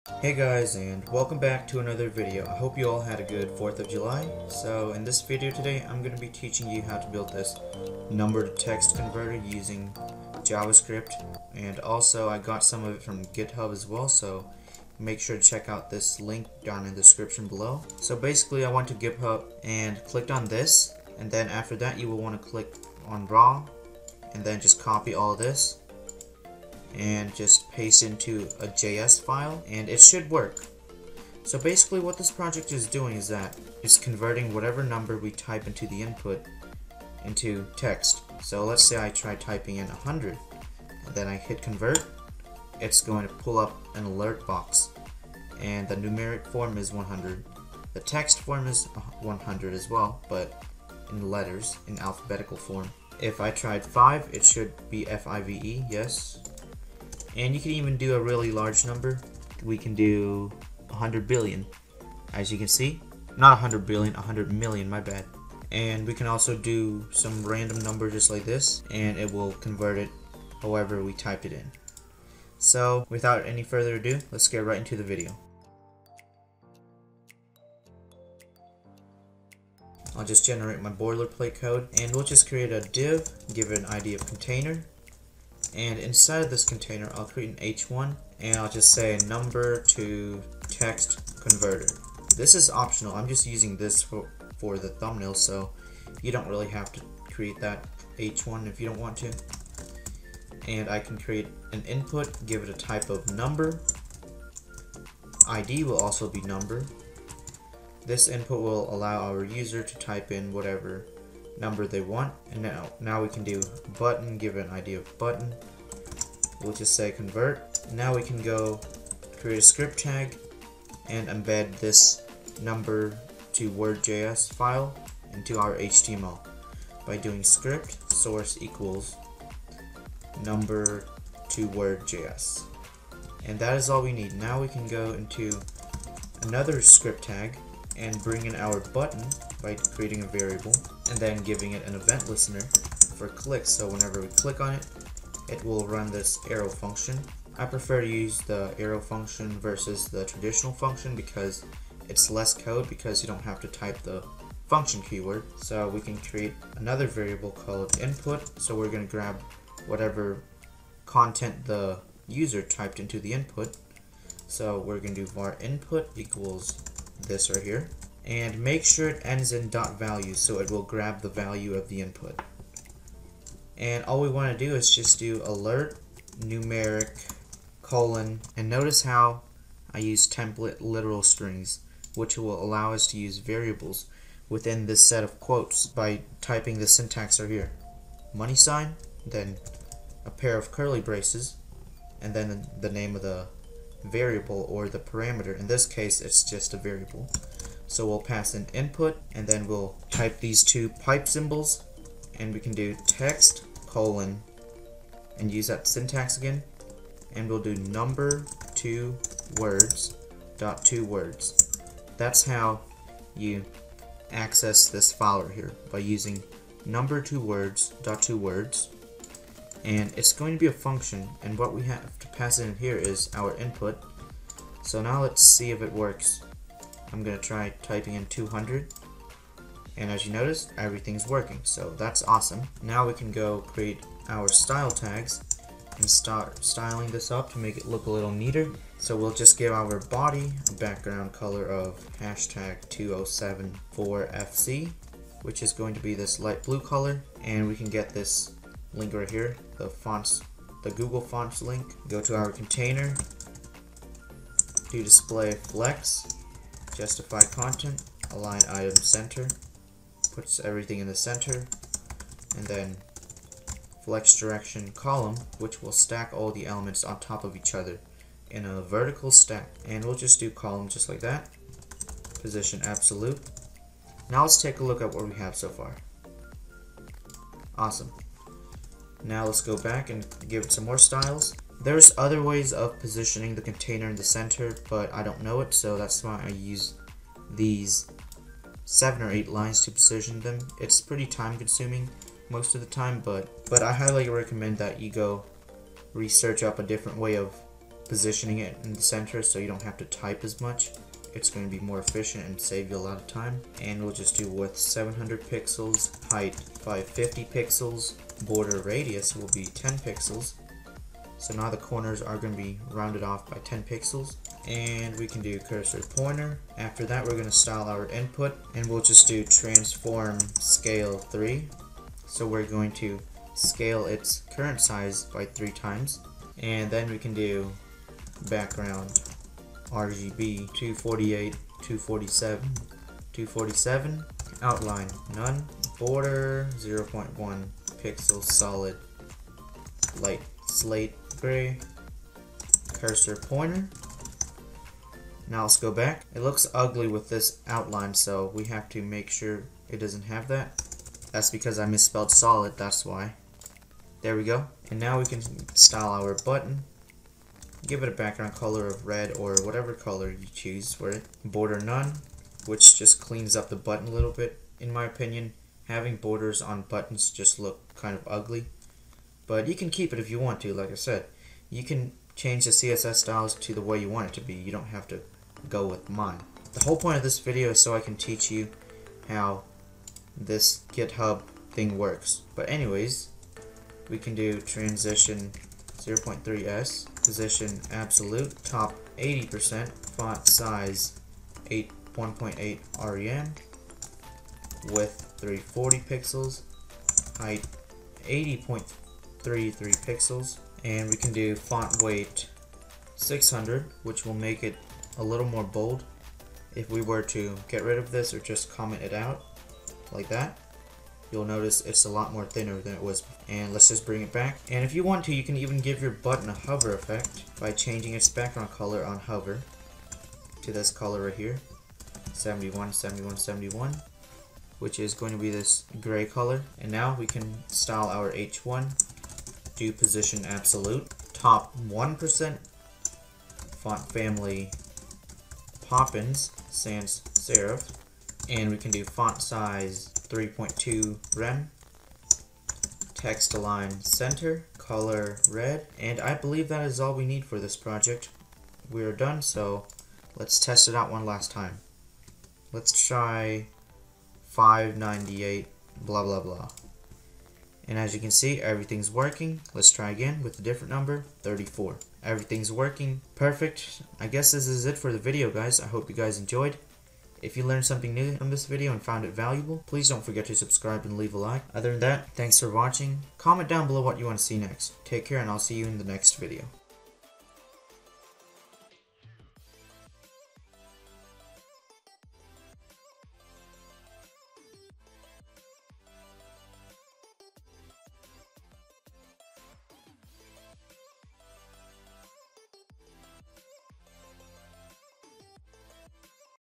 How to convert Numbers to strings/words using JS
Hey guys, this is a fairly simple video to comprehend for most of us. So basically, I found this github link that allowed you to convert numerical numbers into it's corresponding string form which is pretty cool so I hope you guys liked it. I will be showing you some cool projects you can use with what you learned in this video in the next few videos!